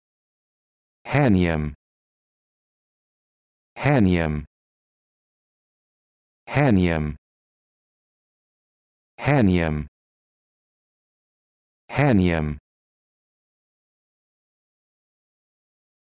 How to Pronounce Hahnium
This video shows you how to pronounce Hahnium